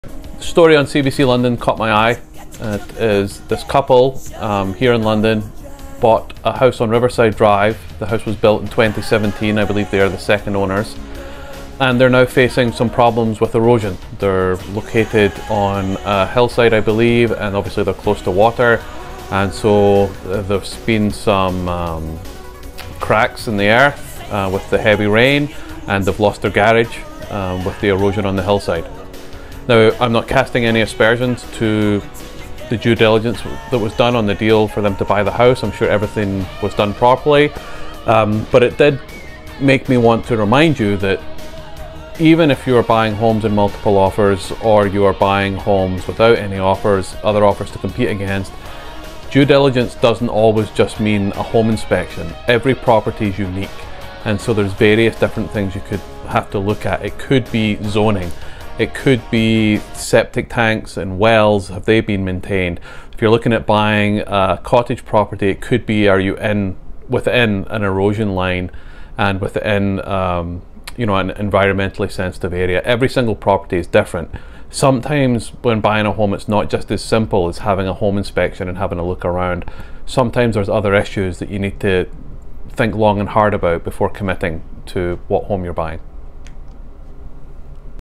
Sometimes A Home Inspection Isn't Enough:
Ensuring Your Home Purchase is a Wise Investment

As a potential homebuyer, you may have attended an open house and had a home inspection conducted, but what if that isn't enough? While a home inspection is an essential step in the home-buying process, it may not reveal all potential issues that could impact your investment in the future. In Ontario, it is crucial to do due diligence before making an offer to purchase, including additional studies and reports beyond a home inspection.